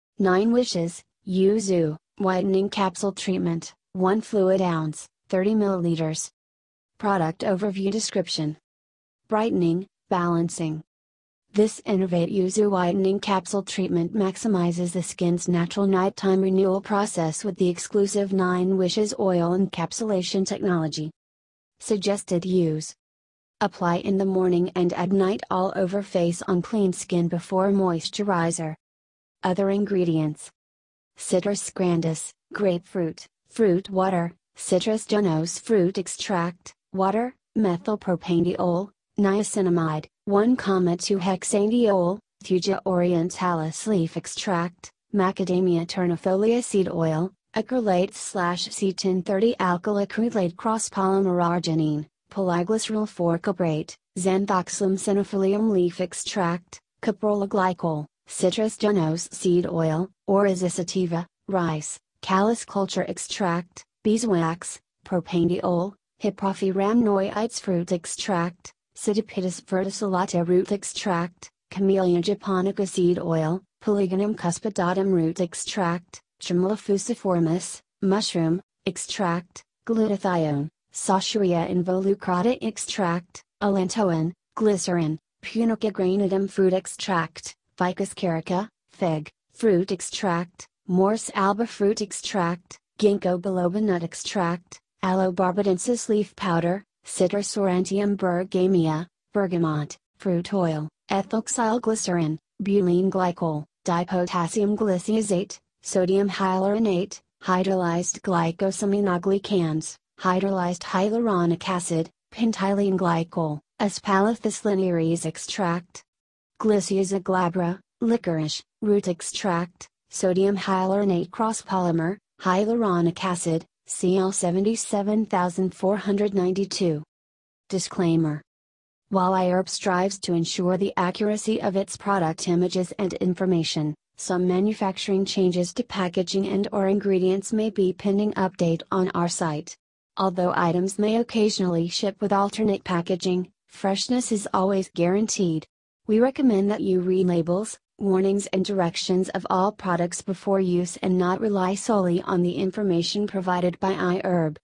9Wishes, Yuzu, Whitening Capsule Treatment, 1 fl oz (30 ml)
9Wishes, Yuzu, Whitening Capsule Treatment, 1 fl oz (30 ml)

Product Overview
Description

    Brightening / Balancing

This innovate Yuzu whitening capsule treatment maximizes the skin's natural night time renewal process with the exclusive 9wishes' oil encapsulation technology.
Suggested Use

Apply in the morning and at night all over face on clean skin before moisturizer.
Other Ingredients